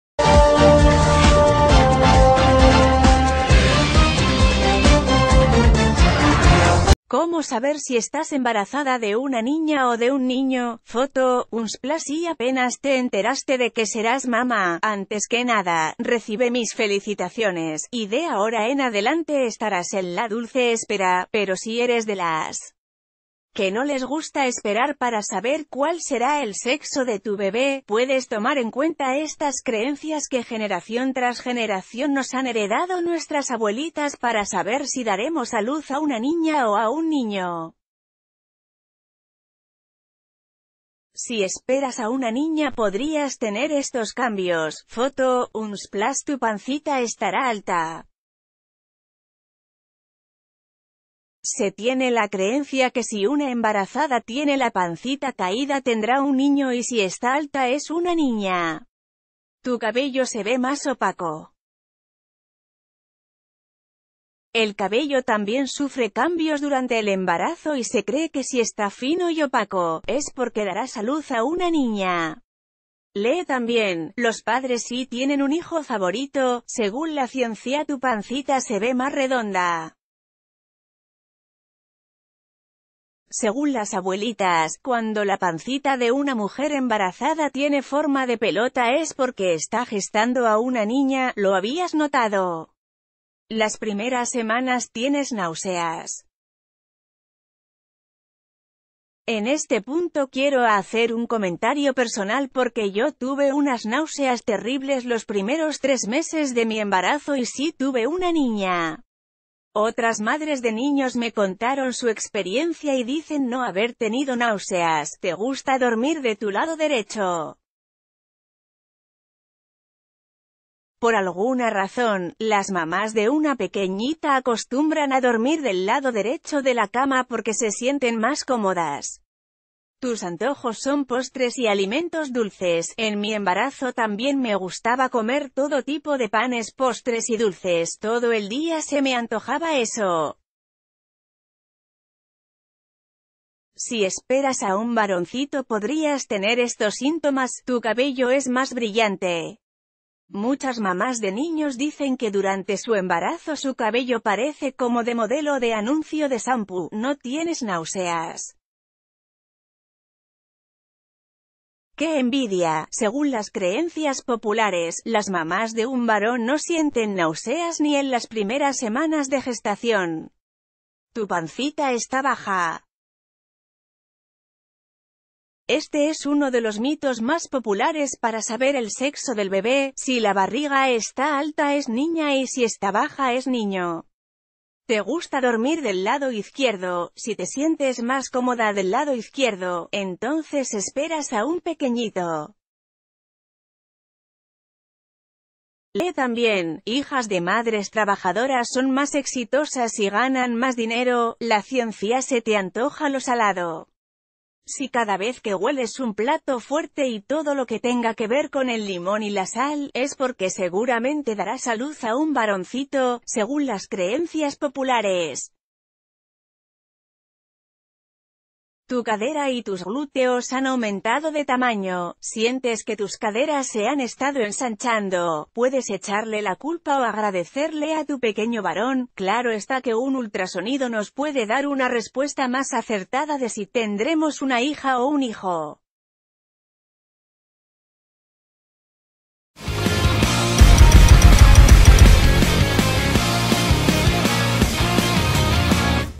Cómo saber si estás embarazada de una niña o de un niño
Cómo saber si estás embarazada de una niña o de un niño | Foto: Unsplash Si apenas te enteraste de que serás mamá, antes que nada, recibe mis felicitaciones, y de ahora en adelante estarás en la dulce espera, pero si eres de las que no les gusta esperar para saber cuál será el sexo de tu bebé, puede...